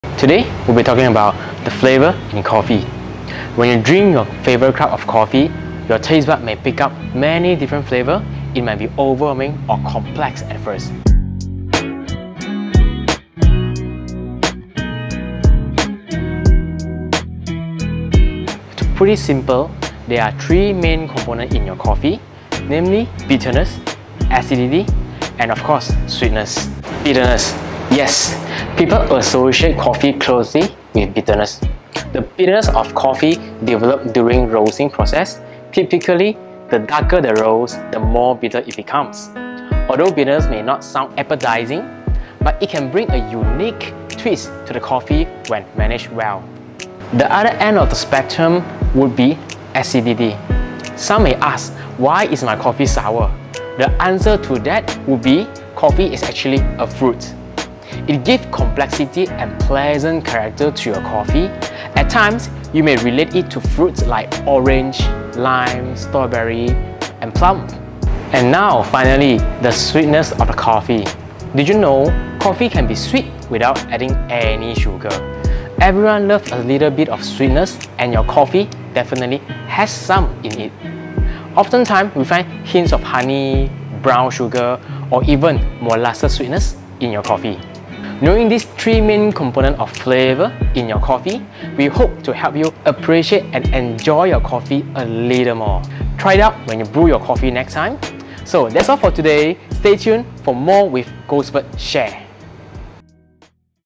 Flavours in Coffee
Does coffee taste just like coffee to you?

There are many factors affecting the flavours in your favourite cup of coffee. The three main components would be bitterness, acidity and sweetness.

Let us share with you more about the flavours in coffee!